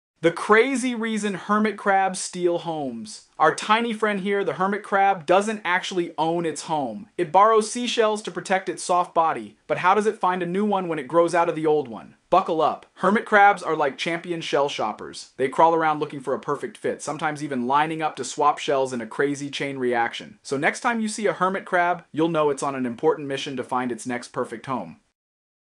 Naked & Afraid?! The SHOCKING Truth About Hermit Crab Shells
Ever wondered why hermit crabs carry shells? It's not just fashion! They're basically homeless without them, facing a scary fate.